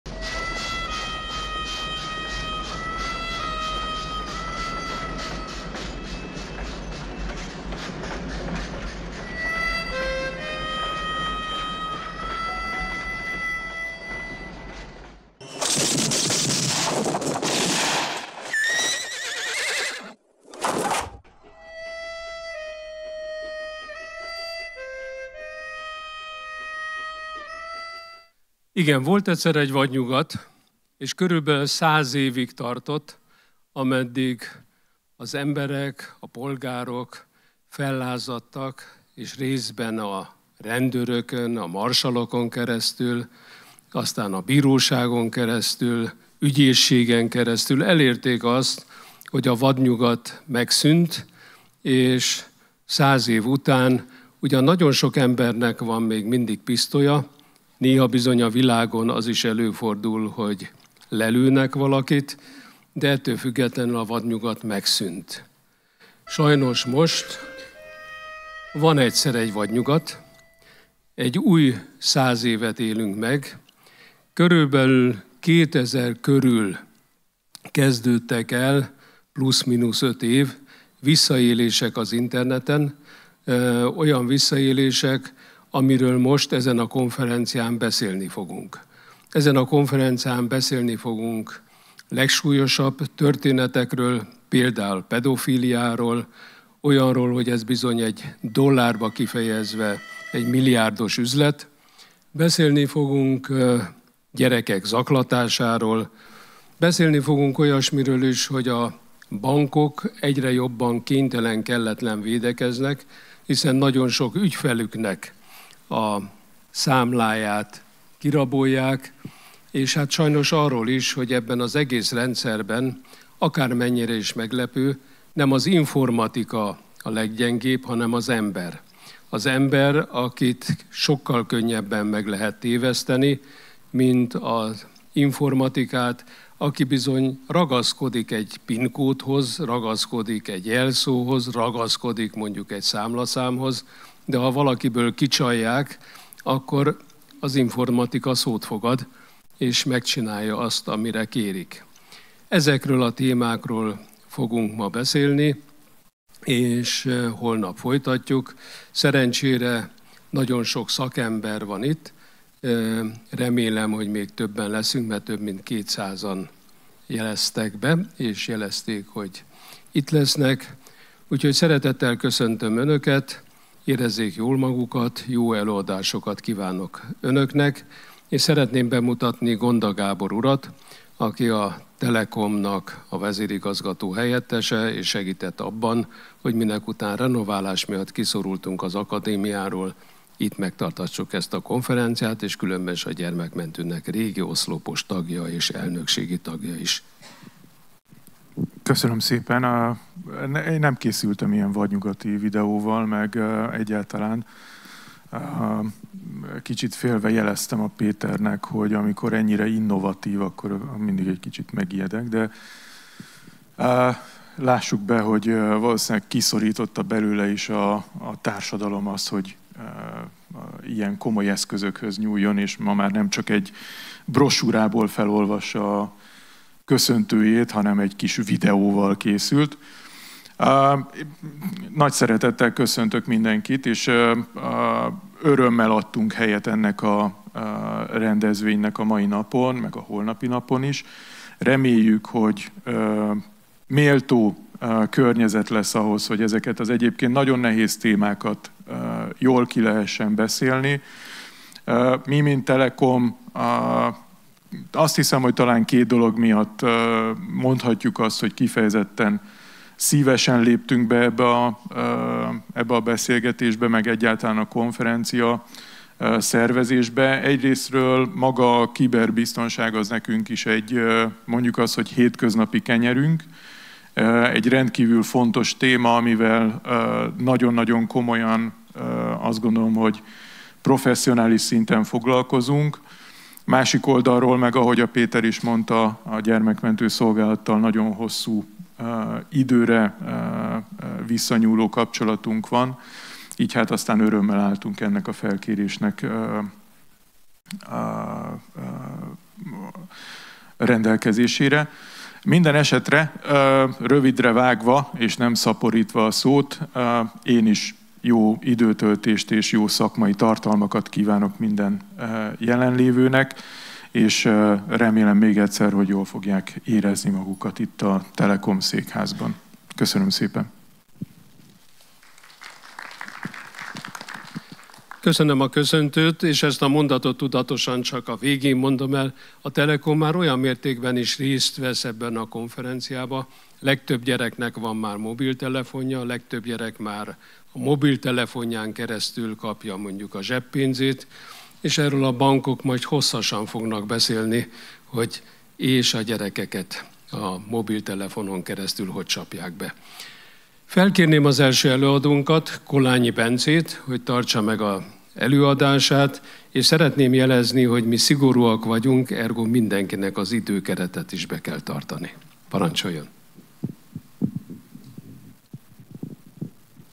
Nyitóbeszéd - Edvi Péter és Gonda Gábor
XIII. Média- és Internetkonferencia
2023. szeptember 18-19. – Magyar Telekom Székház